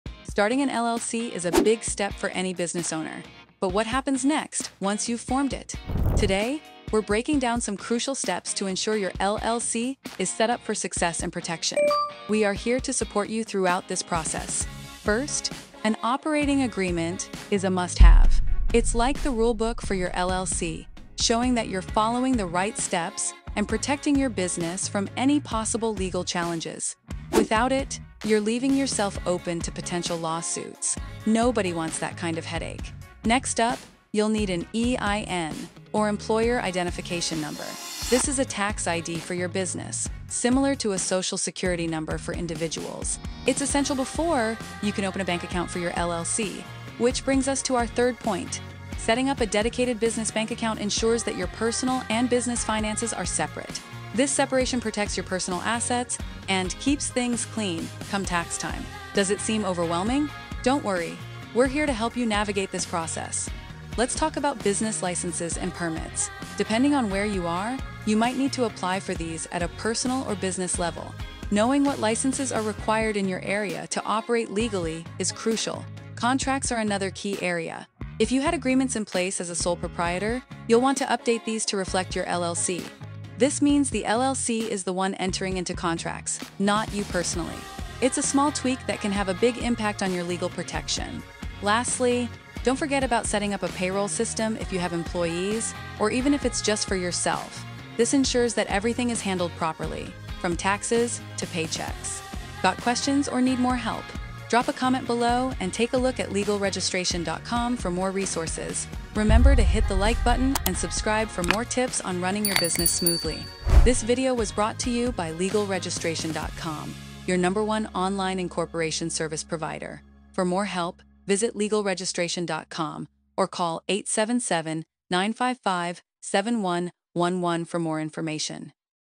What to Do AFTER You Form an LLC - 6 Steps for Success
1. Draft an Operating Agreement
First things first, you need an operating agreement. This document outlines how your LLC will run, who’s in charge of what, and how decisions will be made. It’s not just a formality—it’s your LLC’s rulebook. Even if your state doesn’t require it, having an operating agreement helps protect your business from legal challenges down the road. Without it, you could face lawsuits, and let’s be honest, nobody wants that.

2. Obtain an EIN (Employer Identification Number)
Next, you’ll need to get an Employer Identification Number (EIN). Think of this as your LLC’s social security number for tax purposes. You’ll need this number to open a business bank account, which leads us to our next step.

3. Open a Business Bank Account
Once you’ve got your EIN, set up a dedicated business bank account. This separates your personal finances from your business finances—an essential move for protecting your personal assets and simplifying your taxes. Keeping your business and personal finances separate will also help you maintain your LLC’s limited liability status. Sound overwhelming? Don’t worry, we’re here to help!

4. Apply for Necessary Business Licenses and Permits
Depending on your industry and location, you may need to apply for specific business licenses or permits. This could be anything from a general business license to more industry-specific permits. Make sure you research what’s required in your state or local area to avoid fines or legal trouble. You can also use a service to help with this, saving you time and hassle.

5. Update Contracts
If you had any agreements as a sole proprietor, now’s the time to update them. Make sure all contracts are in your LLC’s name going forward, not your personal name. This simple step can offer significant legal protection by ensuring that your LLC, not you personally, is liable for any business agreements.

6. Set Up a Payroll System
Finally, if you have employees—or even if it’s just you—you’ll need to establish a payroll system. This ensures that taxes, paychecks, and any benefits are handled properly. It’s an important step that keeps your business compliant with tax regulations.